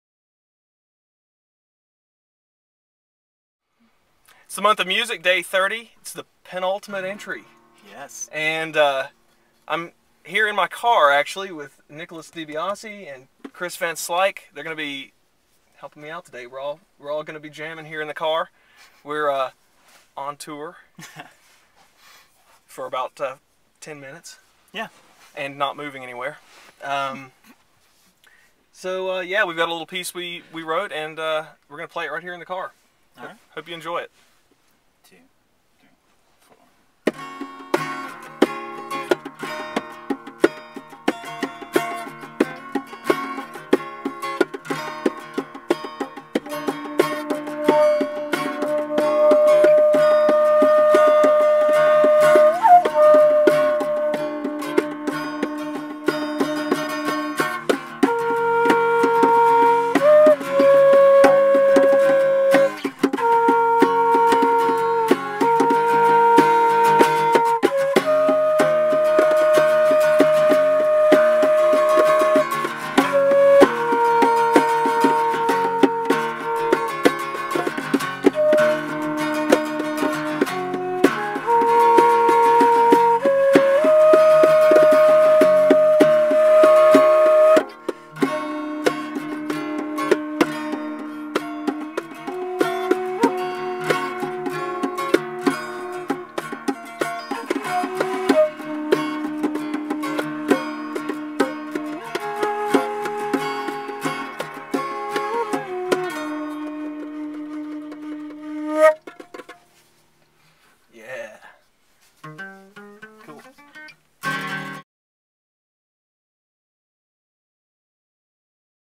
Month of Music - Day 30
For each day in the month of December, I plan to compose a unique, short tune and post it on online. This is the thirtieth one, which Krys VanSlyke, Nicholas DiBiase, and I wrote a few minutes before we performed it for this video.